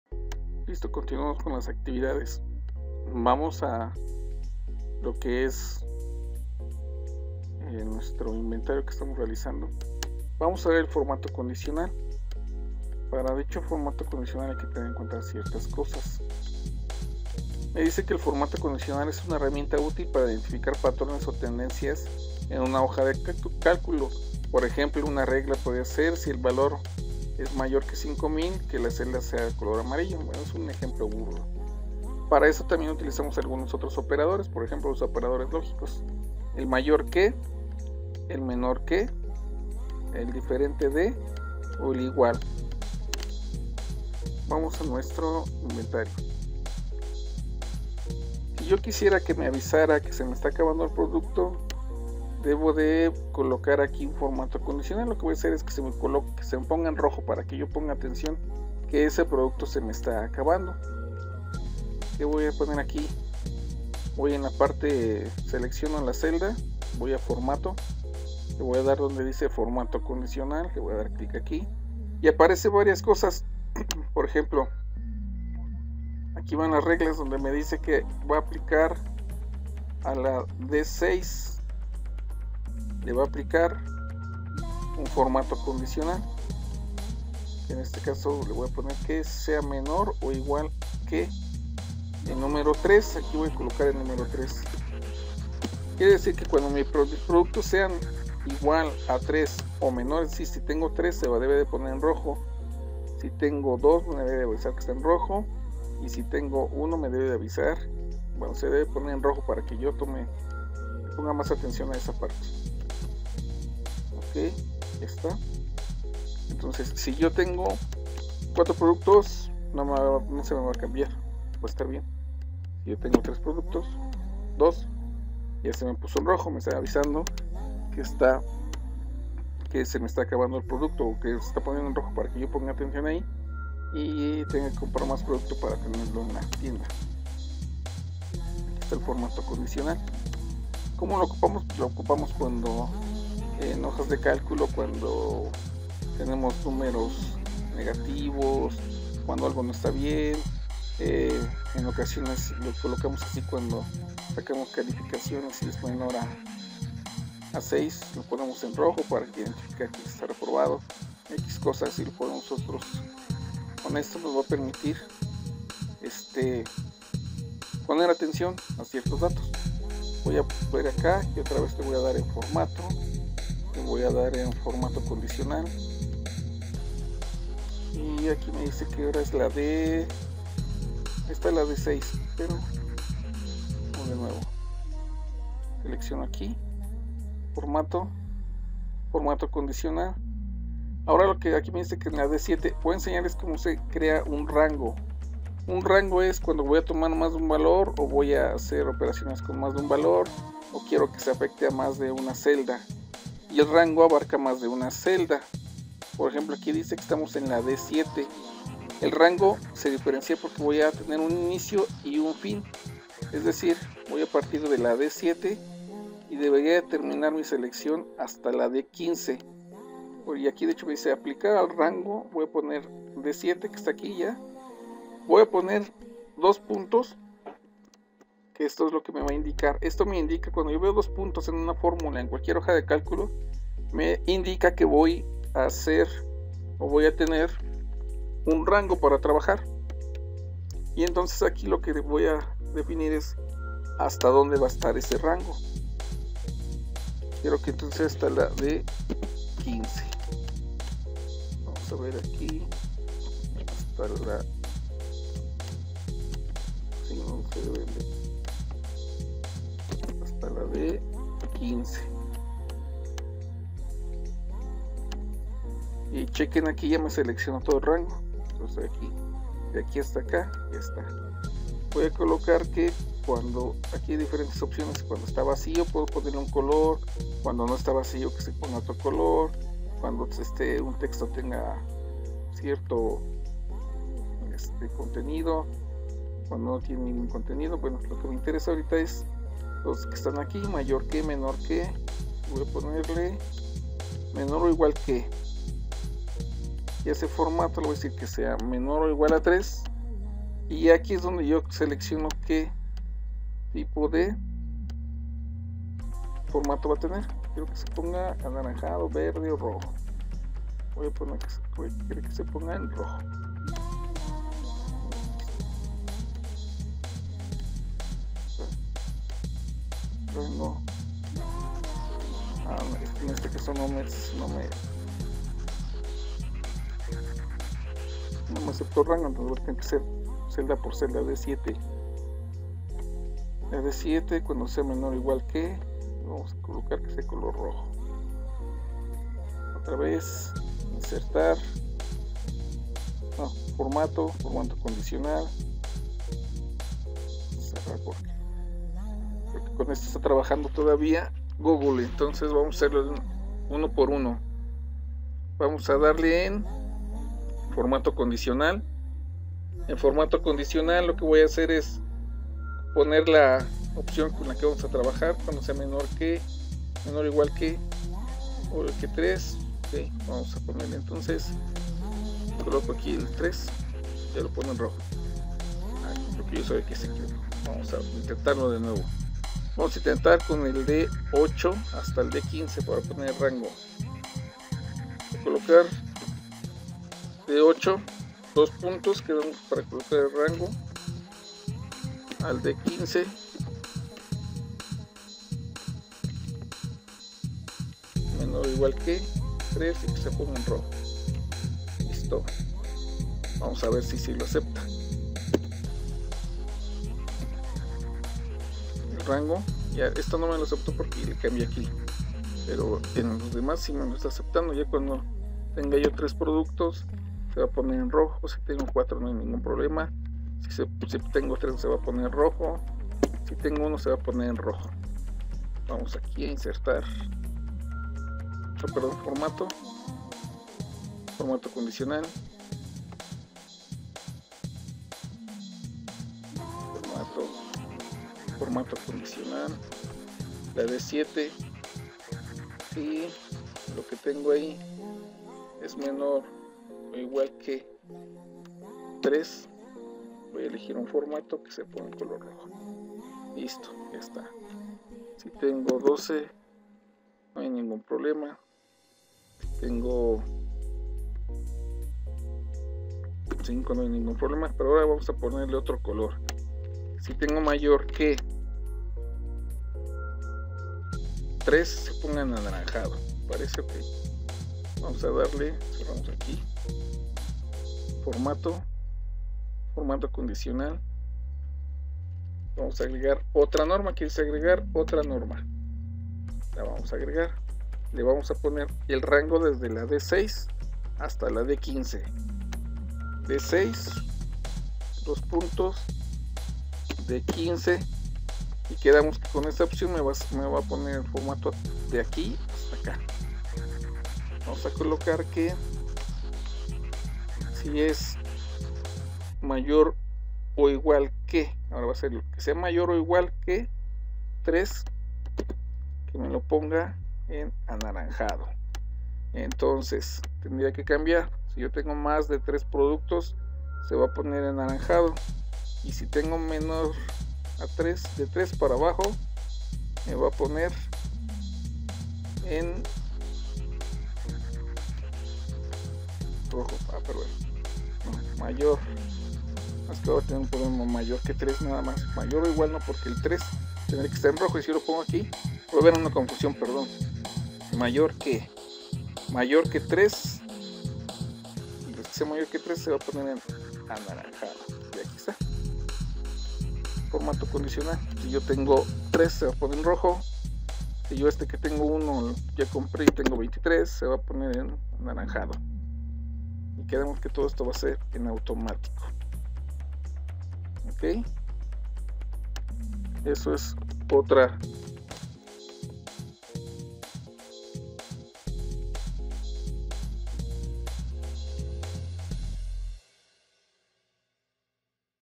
TICs II CBT Hoja de cálculo formato condicional parte 1
Actividades para la materia de TICs II del CBT Nextlalpan.
Contenido central: Uso y manejo de fórmulas, funciones y gráficas en la hoja de cálculo.
Aprendizaje Esperado: Reactiva aprendizajes previos sobre el uso de hoja de cálculo empleado